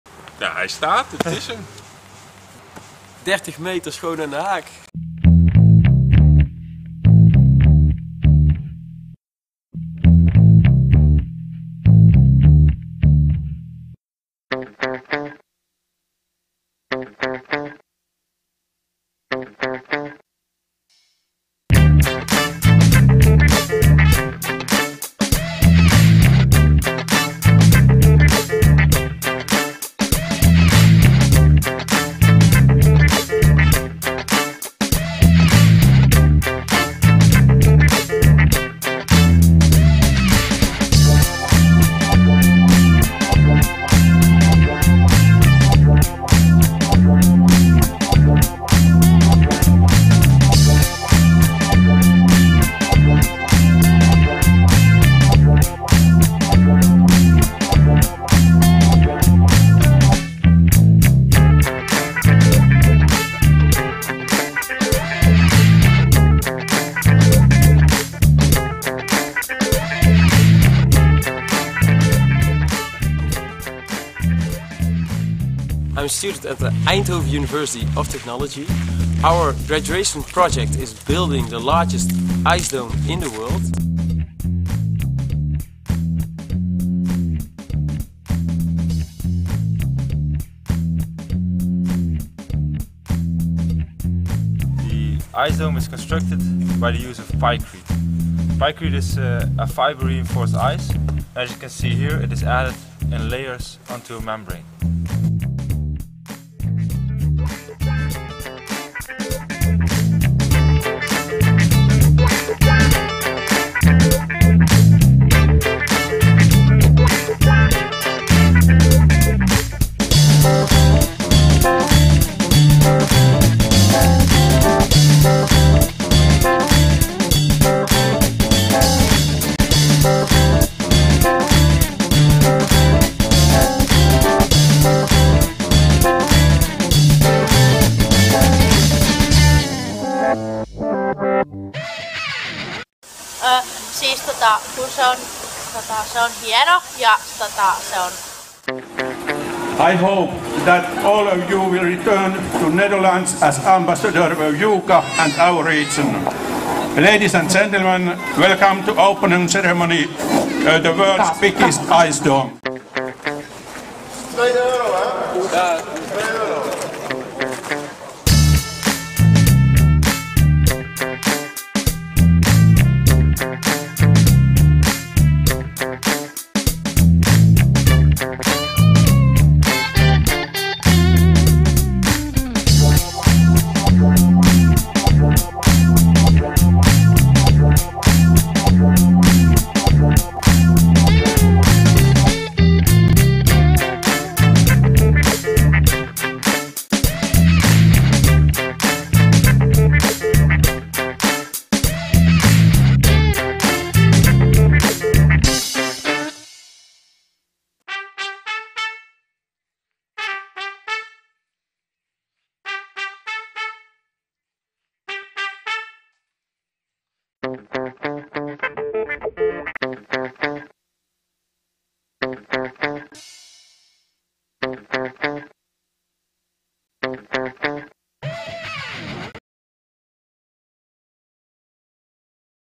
the Pykrete Dome - worlds biggest icedome - a movie by Bart van Overbeeke - Structural Ice
The largest icedome in the world with a span of 30 meters is ready.
The Pykrete Dome was build by students of Eindhoven University of Technology, the Netherlands, together with friends, family, and with enormous support from the community of Juuka, Finland.
The opening on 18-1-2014 was attended by 1500 to 2000 people.

For structural strength, woodfibers (pykrete) are added to the icewater that has been sprayed in layers over an inflatable membrane (balloon).
This sort of strengthened iceconstruction makes new shapes and uses possible.

Finland, Juuka, january 2014

dutch

De grootste ijskoepel ter wereld met een doorsnede van 30 meter is klaar.
De Pykrete Dome is gebouwd door studenten van de Technische Universiteit Eindhoven, met vrienden, familie, en met enorme medewerking van de inwoners van Juuka.
De opening op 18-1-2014 werd bijgewoond door 1500 tot 2000 mensen.

Door water met toegevoegde houtvezels (pykrete) te spuiten over een opblaasbaar membraan (ballon) werd de structuur verstrekt.
Deze vorm van versterkte ijsbouw maakt nieuwe toepassingen en nieuwe vormen mogelijk voor tijdelijke ijsbouw.